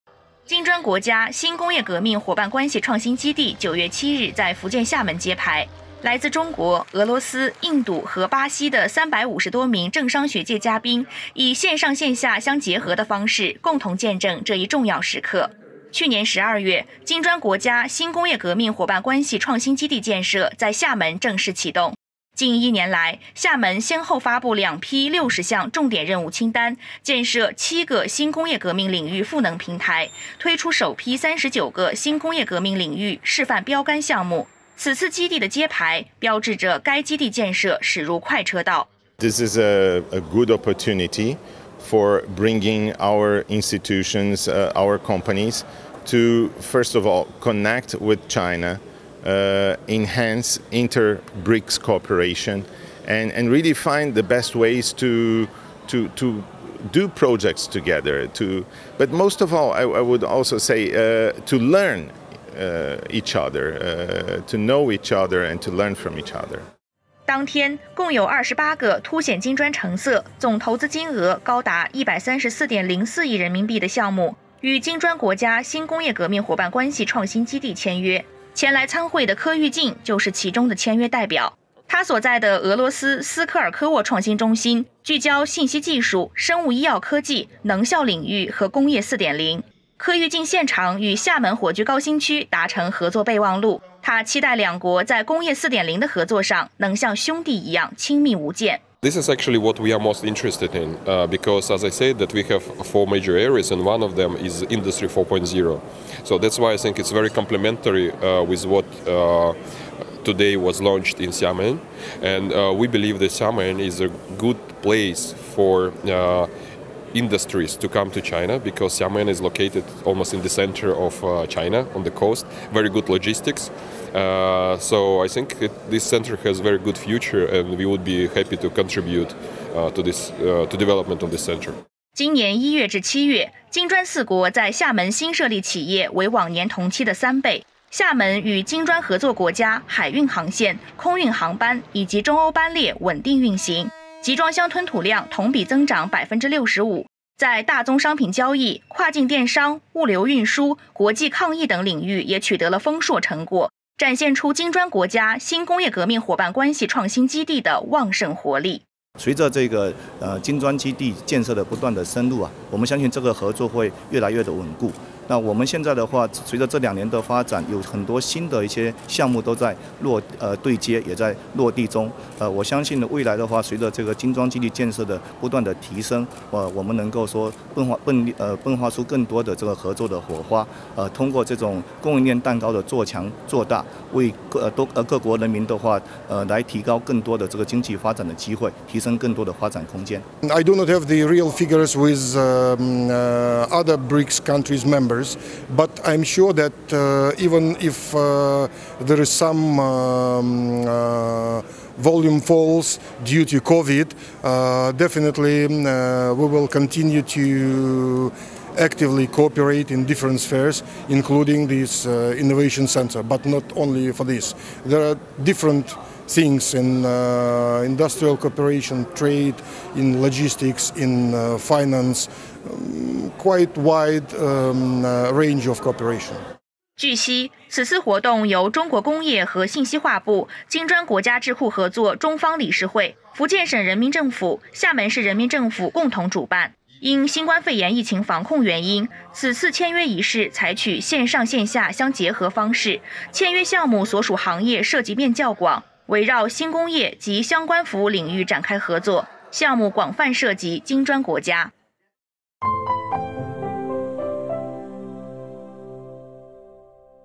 金砖创新基地厦门揭牌 各国代表盼做大供应链“蛋糕”
金砖国家新工业革命伙伴关系创新基地9月7日在福建厦门揭牌，来自中国、俄罗斯、印度和巴西的350多名政商学界嘉宾以线上线下相结合的方式，共同见证这一重要时刻。

去年12月，金砖国家新工业革命伙伴关系创新基地建设在厦门正式启动。近一年来，厦门先后发布两批60项重点任务清单，建设7个新工业革命领域赋能平台，推出首批39个新工业革命领域示范标杆项目，此次基地的揭牌标志着该基地建设驶入快车道。

当天，共有28个凸显金砖成色、总投资金额高达134.04亿人民币的项目与金砖国家新工业革命伙伴关系创新基地签约。前来参会的柯煜靖就是其中的签约代表，他所在的俄罗斯斯科尔科沃创新中心聚焦信息技术、生物医药科技、能效领域和工业4.0，柯煜靖现场与厦门火炬高新区达成《合作备忘录》，他期待两国在工业4.0的合作上能像兄弟一样亲密无间。

今年1月至7月，金砖四国在厦门新设立企业为往年同期的3倍，厦门与金砖合作国家海运航线、空运航班以及中欧班列稳定运行，集装箱吞吐量同比增长65%。在大宗商品交易、跨境电商、物流运输、国际抗疫等领域也取得了丰硕成果，展现出金砖国家新工业革命伙伴关系创新基地的旺盛活力。

据悉，此次活动由中国工业和信息化部、金砖国家智库合作中方理事会、福建省人民政府、厦门市人民政府共同主办，因新冠肺炎疫情防控原因，此次签约仪式采取线上线下相结合方式。签约项目所属行业涉及面较广，围绕新工业及相关服务领域展开合作，项目广泛涉及金砖国家。（李思源 张婷玉）